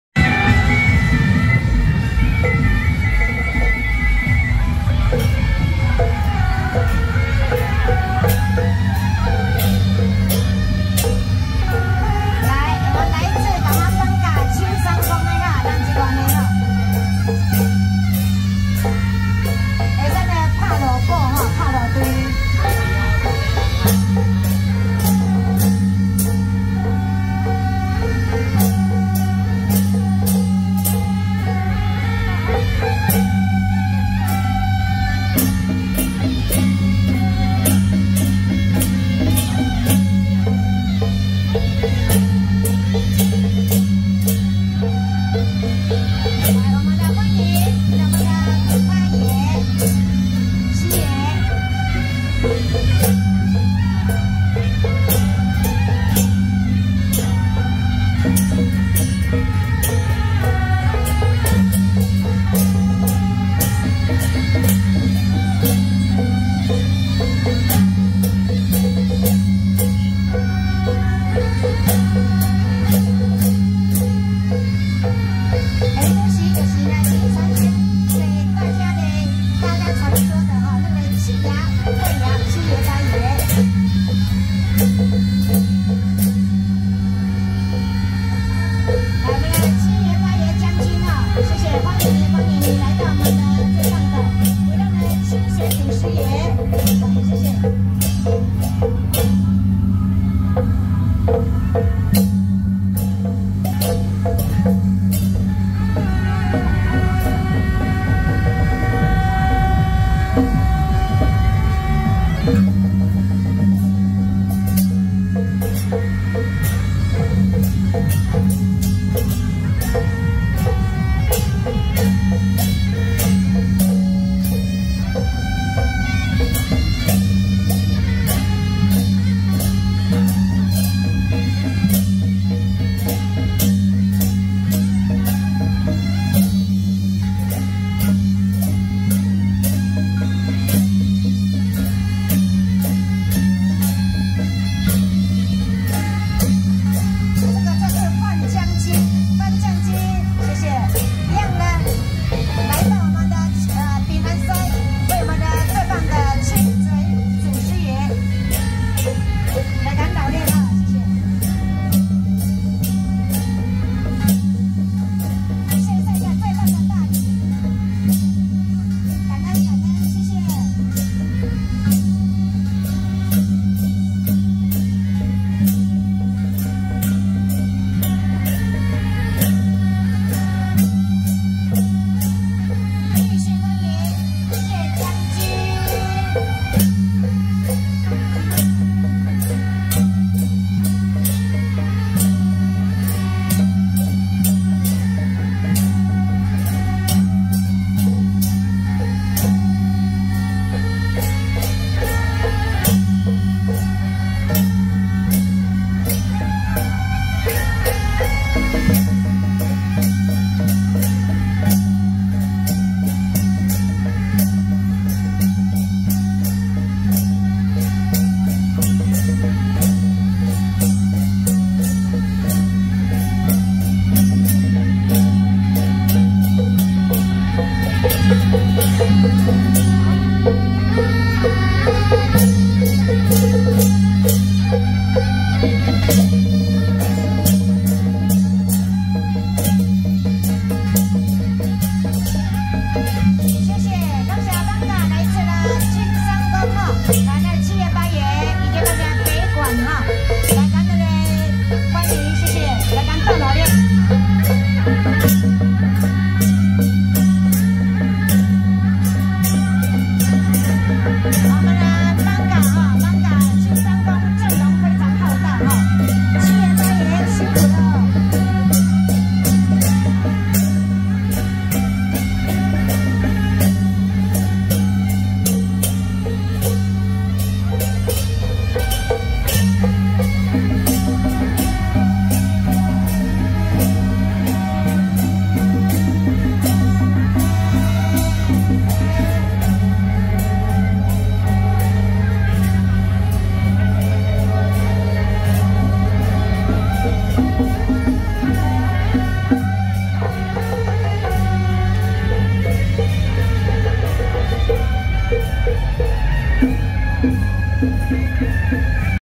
【艋舺青山會+義安社+義英社+鳳音社-神將團】~112淡水清水巖恭迎清水祖師正日遶境(淡水大拜拜)
艋舺青山會+義安社+義英社+鳳音社-神將團